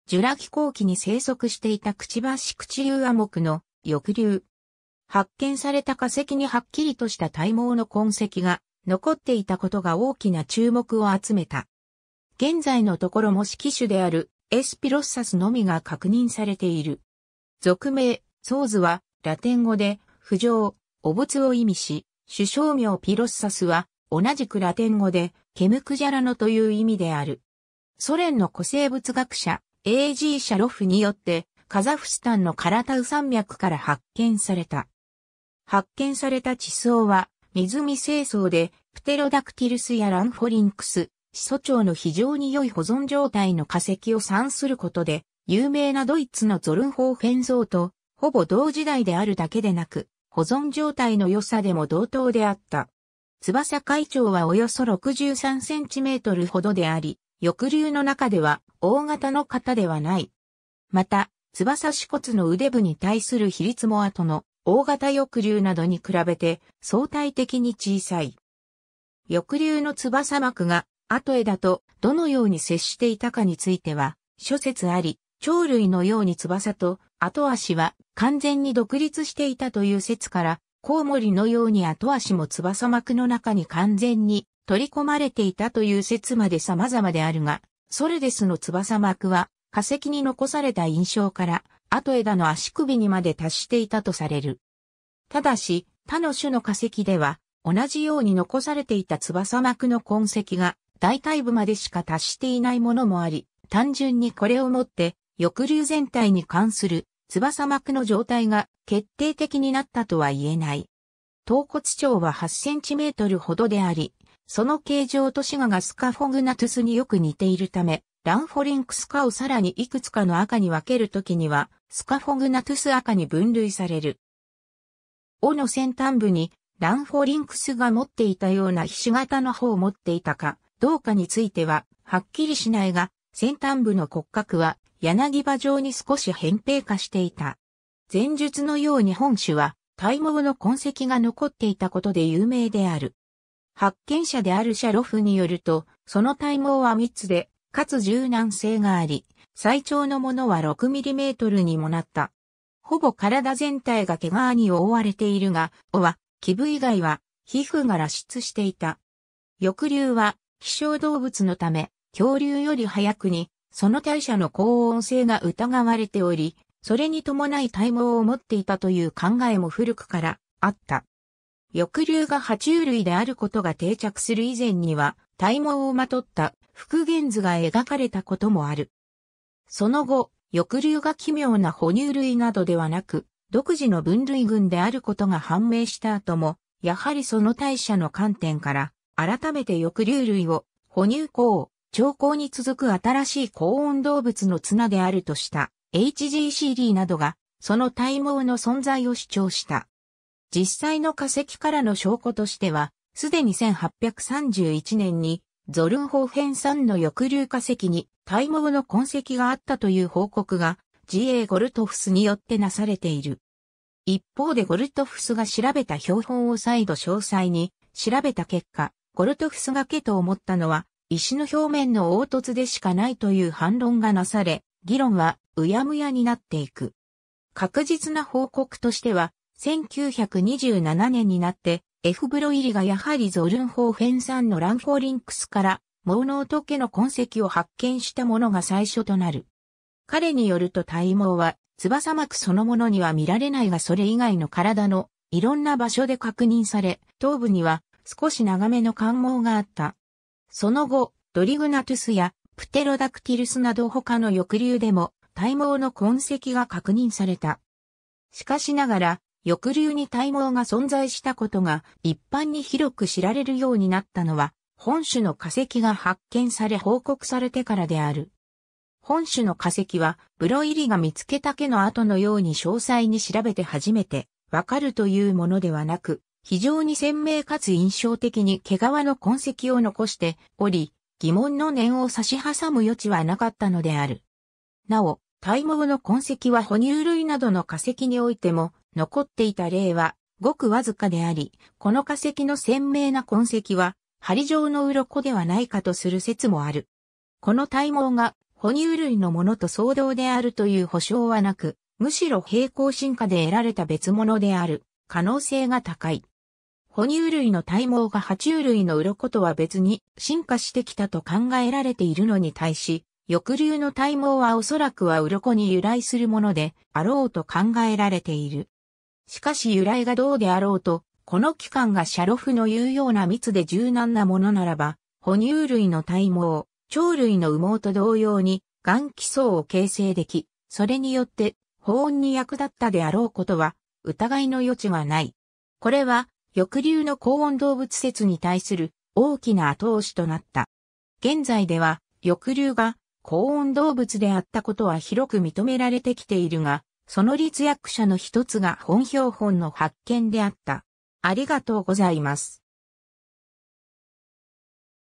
ソルデス
ソルデス (Sordes ) はジュラ紀後期に生息していた嘴口竜亜目の翼竜。
発見された化石にはっきりとした体毛の痕跡が残っていたことが大きな注目を集めた。
現在のところ模式種であるS. pilosus のみが確認されている。
 属名Sordes はラテン語で「不浄・汚物」を意味し、種小名pilosus は同じくラテン語で「毛むくじゃらの」という意味である。
 ソ連の古生物学者 A.G.シャロフ (Aleksandr Grigorevich Sharov) によって、カザフスタンのカラタウ山脈（天山山脈北西麓）から発見された。
発見された地層は湖成層で、プテロダクティルスやランフォリンクス、始祖鳥の非常によい保存状態の化石を産することで有名なドイツのゾルンホーフェン層とほぼ同時代であるだけでなく、保存状態の良さでも同等であった。
 翼開長はおよそ63センチメートル程であり、翼竜の中では大型の方ではない。
また、翼指骨の腕部に対する比率も後の大型翼竜などに比べて相対的に小さい。
 翼竜の翼膜が後枝とどのように接していたかについては諸説有り、鳥類のように翼と後肢は完全に独立していたという説から、コウモリのように後肢も翼膜の中に完全に取り込まれていたという説まで様々であるが、ソルデスの翼膜は化石に残された印象から後枝の足首にまで達していたとされる。
ただし、他の種の化石では同じように残されていた翼膜の痕跡が大腿部までしか達していない物もあり、単純にこれを持って翼竜全体に関する翼膜の状態が決定的になったとは言えない。
 頭骨長は8センチメートルほどであり、その形状と歯牙がスカフォグナトゥスによく似ているため、ランフォリンクス科をさらにいくつかの亜科に分けるときにはスカフォグナトゥス亜科に分類される。
 尾の先端部にランフォリンクスが持っていたような菱形の帆を持っていたかどうかについてははっきりしないが、先端部の骨格は柳葉状に少し扁平化していた。
 前述のように本種は体毛の痕跡が残っていたことで有名である。
発見者であるシャロフによると、その体毛は密でかつ柔軟性があり、最長の物は6mmにもなった。
ほぼ体全体が毛皮に覆われているが、尾は基部以外は皮膚が裸出していた。
 翼竜は飛翔動物のため、恐竜より早くにその代謝の恒温性が疑われており、それに伴い体毛を持っていたという考えも古くからあった。
翼竜が爬虫類であることが定着する以前には、体毛をまとった復元図が描かれたこともある。
（ただしそれは、この飛翔動物が哺乳類であるとの考えによるものであり、現在の視点から見ると必ずしも先見の明があったとは言えない） その後、翼竜が奇妙な哺乳類などではなく独自の分類群であることが判明した後も、やはりその代謝の観点から、あらためて翼竜類を哺乳綱・鳥綱に続く新しい恒温動物の綱であるとした H.G.シーリー (Harry Govier Seeley) などが、その体毛の存在を主張した。
 実際の化石からの証拠としては、既に1831年にゾルンホーフェン産の翼竜化石に体毛の痕跡があったという報告が G.A.ゴルトフス (Georg August Goldfuß) によって成されている。
一方でゴルトフスが調べた標本を再度詳細に調べた結果、ゴルトフスが毛と思ったのは石の表面の凹凸でしかないという反論がなされ、議論はうやむやになっていく。
 確実な報告としては、1927年になって、F.ブロイリ (Ferdinand Broili) がやはりゾルンホーフェン産のランフォリンクスから毛嚢と毛の痕跡を発見したものが最初となる。
彼によると体毛は翼膜そのものには見られないがそれ以外の身体のいろんな場所で確認され、頭部には少し長めの冠毛があった。
その後、ドリグナトゥスやプテロダクティルスなど他の翼竜でも体毛の痕跡が確認された。
 しかしながら、翼竜に体毛が存在したことが一般に広く知られるようになったのは、本種の化石が発見され報告されてからである。
本種の化石は、ブロイリが見つけた毛の跡のように詳細に調べて初めてわかるという物ではなく、非常に鮮明かつ印象的に毛皮の痕跡を残しており、疑問の念を差し挟む余地はなかったのである。
 尚、体毛の痕跡は哺乳類などの化石においても残っていた例は極僅かであり、この化石の鮮明な痕跡は針状の鱗ではないかとする説もある。
 この体毛が哺乳類のものと相同であるという保証はなく、むしろ平行進化で得られた別物（相似器官）である可能性が高い。
哺乳類の体毛が爬虫類の鱗とは...